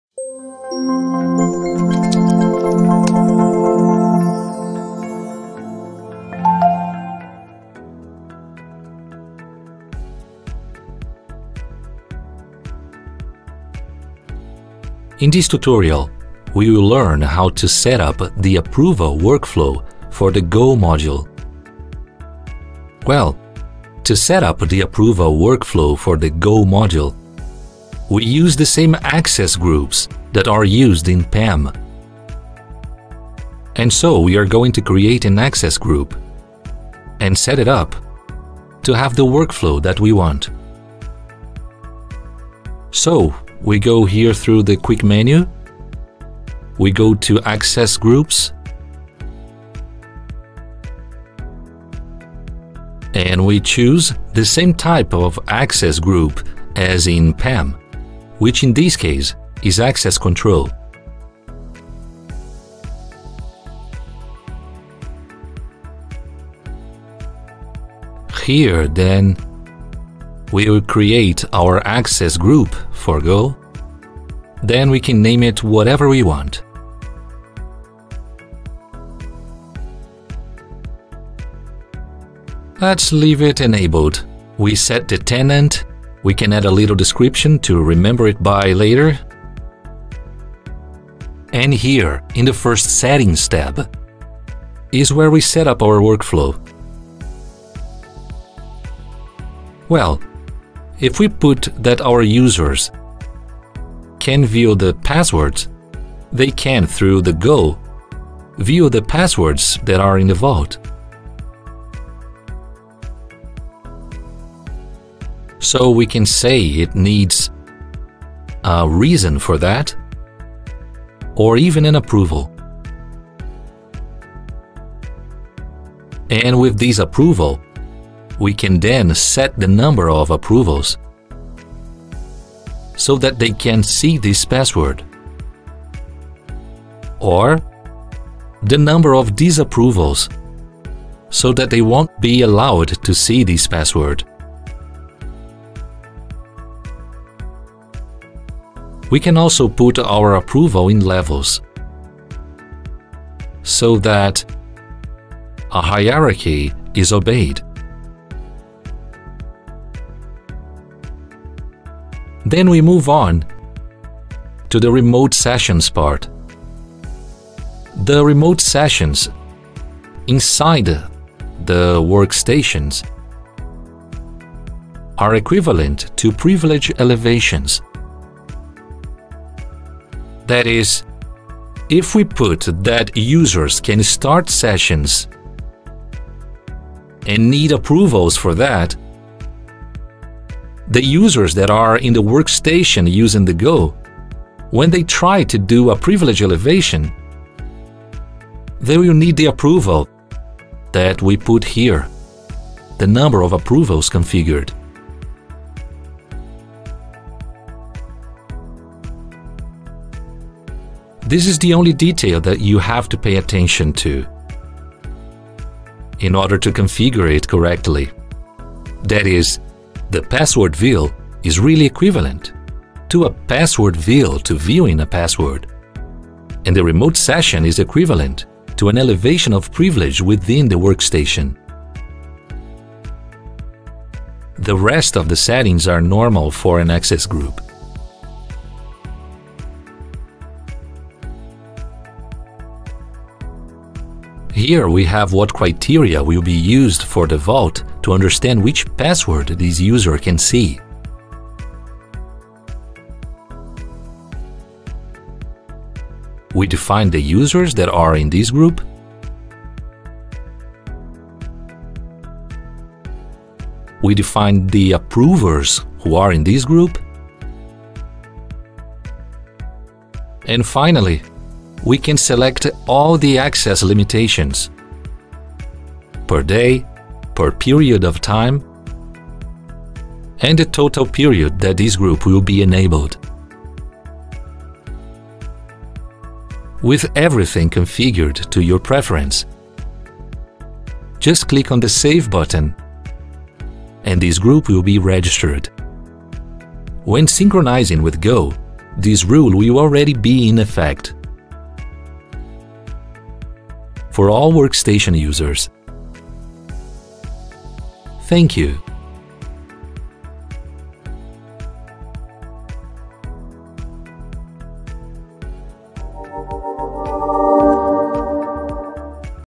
How to configure the approval workflow for senhasegura.go
In this video you will check how to configure the approval workflow for senhasegura.go:

1. In the Quick Menu click on Access Group.
2. Define a name for the access group
3. In the tab Settings there are three sections, Password preview settings, Remote sessions settings, and Access request settings.
4. In Password preview settings,  select what permissions this group will have, such as view password, require a reason to view the password, or require approval to view a password. Set up the number of ‘Approval required for viewing’, “Disapproval required to cancel”
5. In Remote sessions settings select what action the users can perform as ‘Users can start session’, ‘Require reason to start session’, ‘Require approval to start session’.
6. In the tab Criteria select what kind of Site, Device Type, and Credential Type the users can view.
7. In the tab Users, select the user of the access group.
8. In the tab Approvers, select the approvers of the access group.
9. In the tab Access Limitation, select the Access Permission Days, Access Permission Time, and Access Permission Period.
10. Click in Save to finish the configuration.